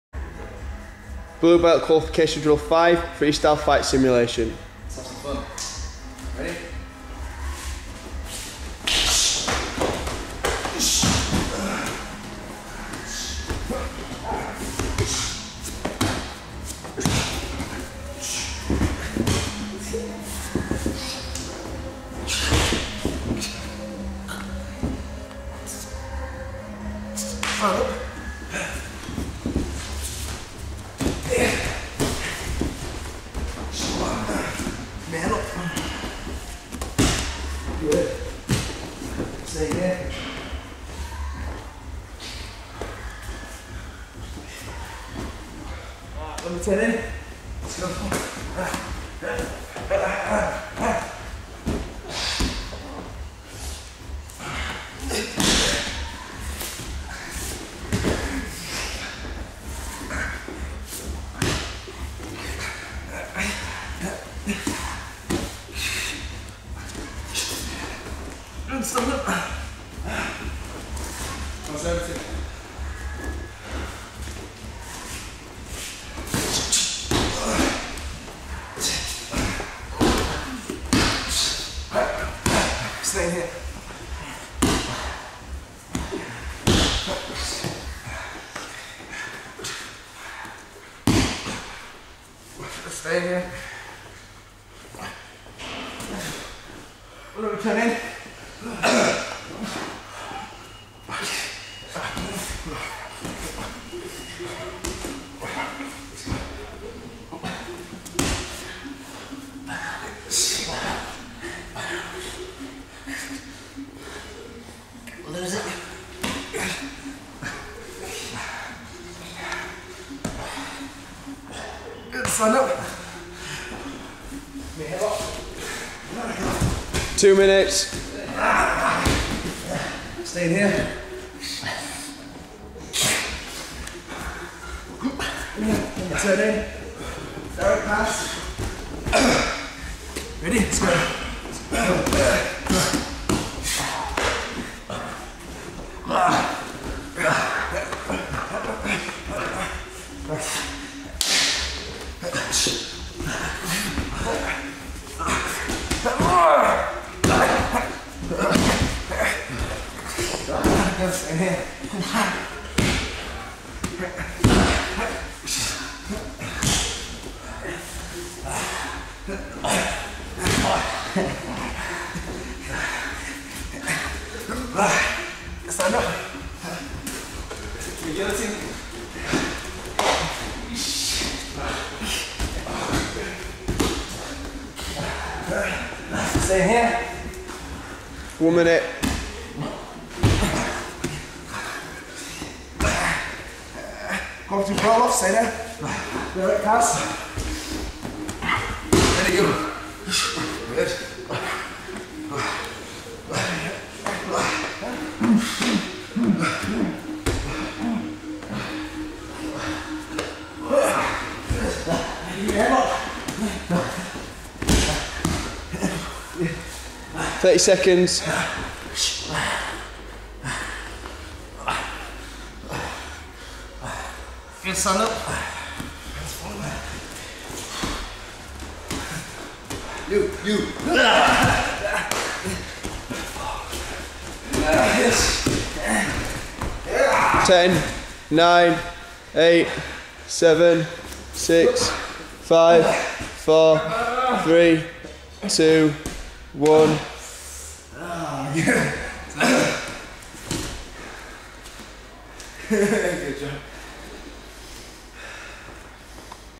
Wayne Thompson Blue Belt Qualificaion Drill 5
UK GRACIE JIU-JITSU (UKGJJ) student  Wayne Thompson's Official Gracie Jiu-Jitsu Blue Belt Qualification Drill 5. BBQD5 consists of a freestyle fight simulation using techniques from the 'Gracie Combatives' program. The 'Bad Guy' plays the roll of un 'unskilled' attacker in a unscripted street fight simulation and feeds the student key indicators in his behavior & movement which he must then react to with the correct reaction. UKGJJ is Europes only certified Gracie Jiu-Jitsu training centre more information can be found at: